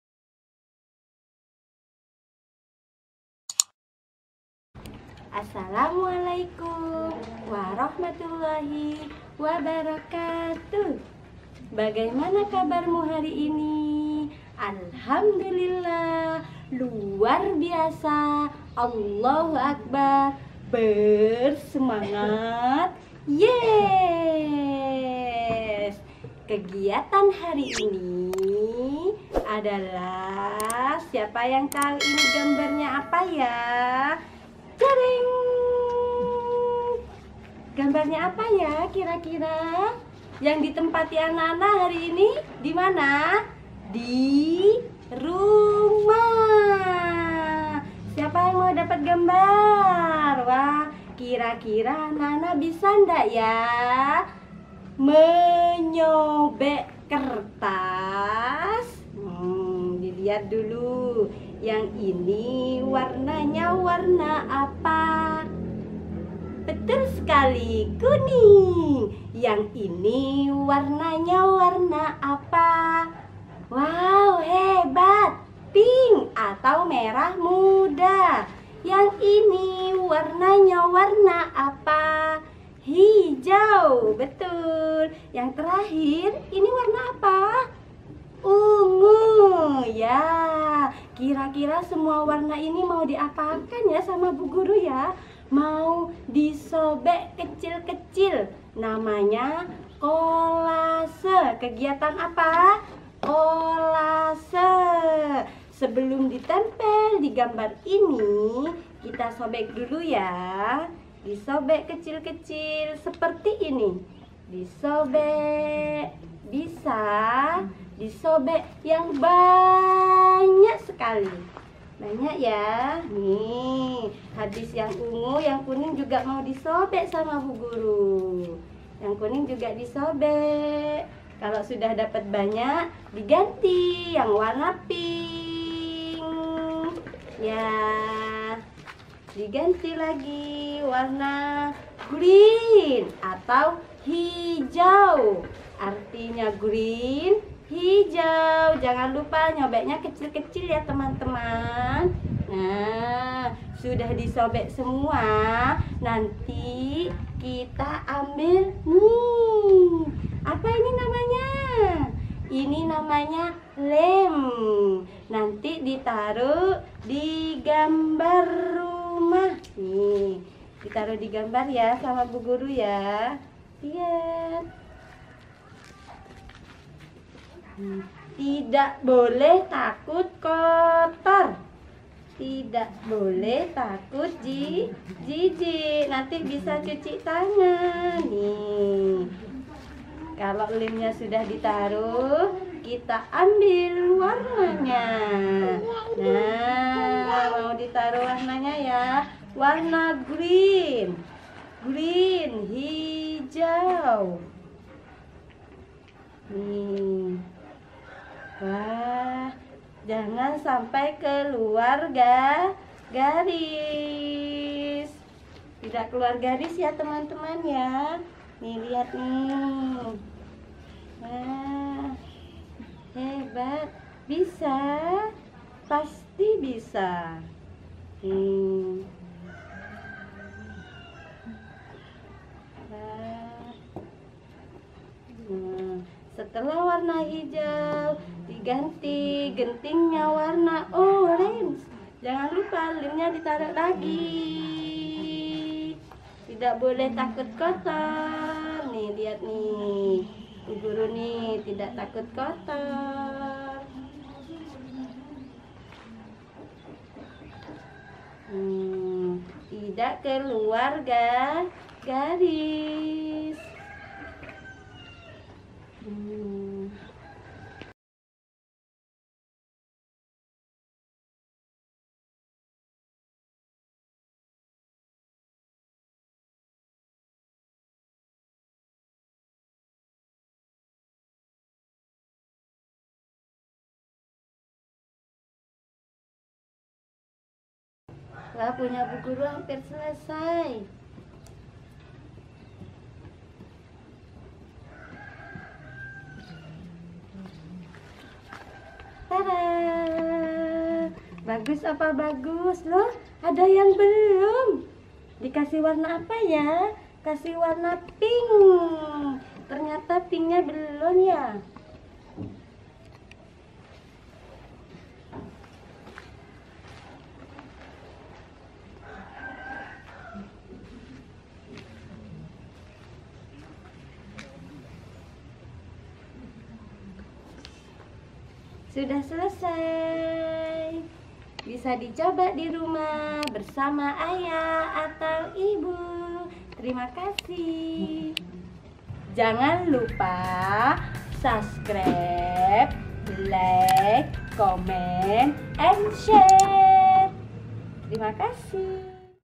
Video Pembelajaran Kolase Tema Lingkunganku | BDR | Belajar Daring Paud
Video ini dibuat untuk memenuhi penugasan materi Praktek Pembuatan  Pelatihan Teknis Pembelajaran Guru PAUD  Secara Daring Jambuan Antirogo Jember

Berkhidmat Untuk Pendidikan

Semoga mereka semuadijadikan anak yang sholeh dan sholehah berbakti kepada orang tua dan gurunya
Amiin amiin amiin ya robbal 'alamiin

Semoga Bermanfaat.

Jazakumullaah Ahsanal Jaza'